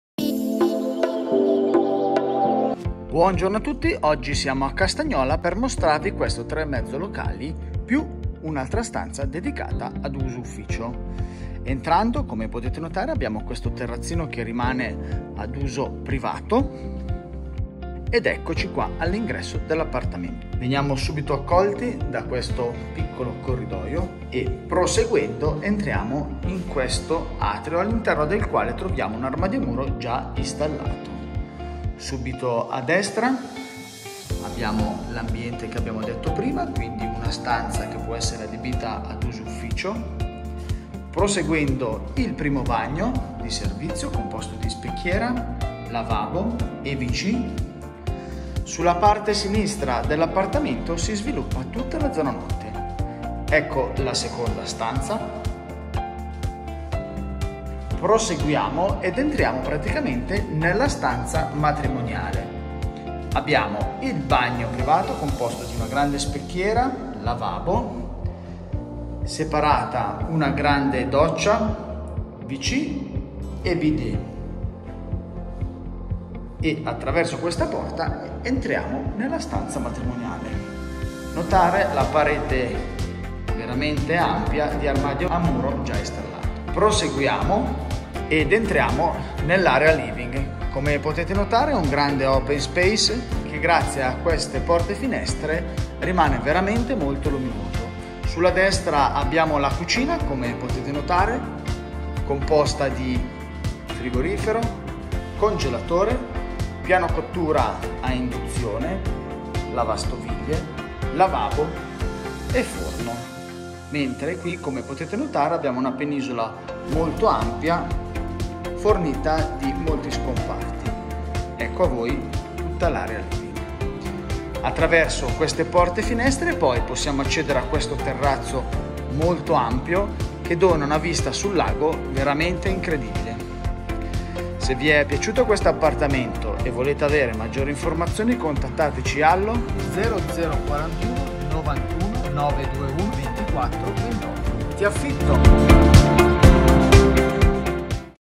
GRANDE 3.5 CON UFFICIO E VISTA LAGO A CASTAGNOLA
Siamo a Castagnola per mostrarti un bellissimo appartamento di 110 mq totali.

Se il tuo sogno è sempre stato quello di vivere in una casa con una VISTA MOZZAFIATO, ma non hai avuto il tempo e l’occasione per trovare la casa ideale per voi, sei nel posto giusto.

Questo delizioso appartamento è ubicato in una ZONA BEN SERVITA dagli istituti scolastici e collegata grazie ai trasporti pubblici al rinomato comune di Lugano.

È ideale per una coppia o una piccola famiglia in cerca di un nido accogliente in cui vivere e lavorare.

Questo appartamento si trova al PRIMO PIANO di uno stabile di 6 piani.
Accediamo alla porta d’ingresso tramite un grande TERRAZZO PRIVATO.

Accedendo in un corridoio con APPENDIABITI che ci conduce nell’atrio - arredato con un ARMADIO A MURO – che collega tutte le zone dell’abitazione.

Sulla destra è presene un locale PERFETTO COME UFFICIO per chi lavora in home-working.
IL BAGNO DI SERVIZIO è presente subito dopo ed è moderno e di recente rinnovo.

Continuando verso sinistra troviamo le altre DUE CAMERE: una singola ed una padronale.
Quest’ultima ospita UN BAGNO EN-SUITTE dalle linee moderne con doccia, sanitari e grande lavabo ed è dotata di un grandissimo armadio a muro.

In ultimo ci dirigiamo verso LA ZONA GIORNO, incredibilmente illuminata da grandissime vetrate che prospettano un panorama incantevole.

Queste, oltre a rendere luminosissimo l’ambiente, permettono l’accesso al TERRAZZO ampio ed abitabile dotato di tende parasole.
Appena messo piede sul balcone resterai sbalordito: la vista, da qui, è incredibile.

Questo 4,5 locali infatti si trova VICINO AL LAGO e potrai osservare di giorno in giorno i colori e le sfumature che esso assume.

La zona living è suddivisa NEL SALOTTO, dove potrai mettere un grande divano e dalla cucina aperta.

LA CUCINA è NUOVA ed offre al soggiorno la sua moderna isola.
Si completa di forno, piastre, lavandino, frigorifero e tanti mobili per riporre gli oggetti.

Il riscaldamento è servito da termosifoni per un calore uniforme e asciutto.

L’affitto netto è di 2'360 franchi e le spese condominiali sono pari a 460 franchi.

È disponibile 1 BOX AUTO al costo di 180 franchi mensili.

La garanzia affitti richiesta è pari a tre mensilità anche tramite assicurazione.

Per restare anche tu senza fiato dal balcone di questo appartamento con una bellissima vista sul Lago, contattaci al oppure al .

TiAffitto